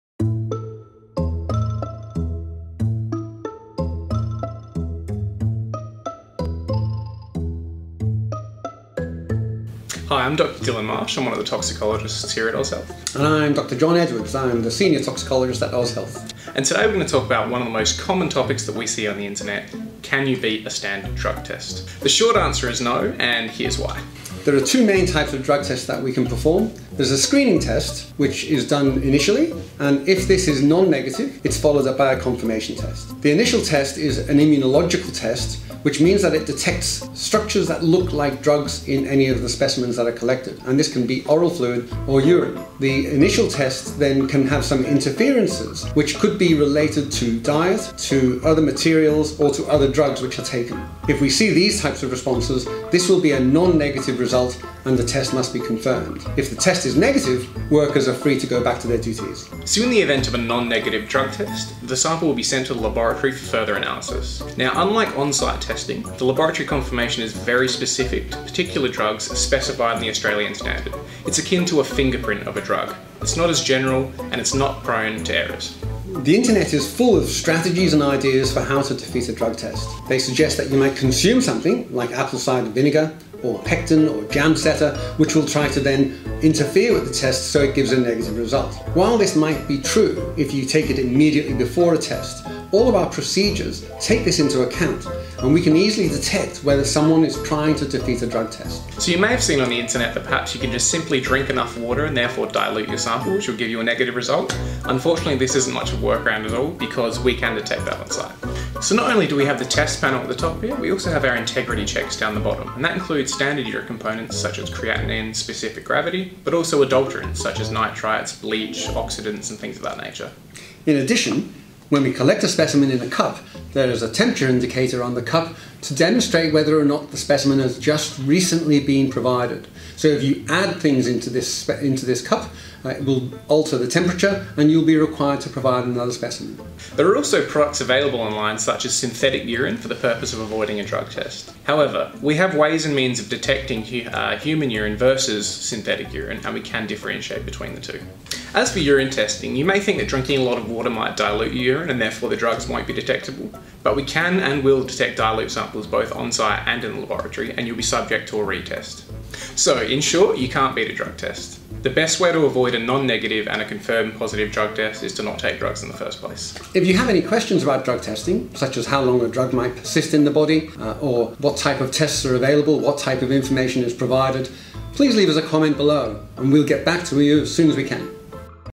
Can you beat a drug test? A Toxicologist Explains | AusHealth
This week, our Toxicologists answered one of the most searched questions on the internet - is it possible to beat a drug test?

The answer is no - and here’s why.

AusHealth’s Dr John Edwards and Dr Dylan Marsh explain some of the most common misconceptions about the drug testing procedure in Australia, and debunk some of the popular methods circulating online.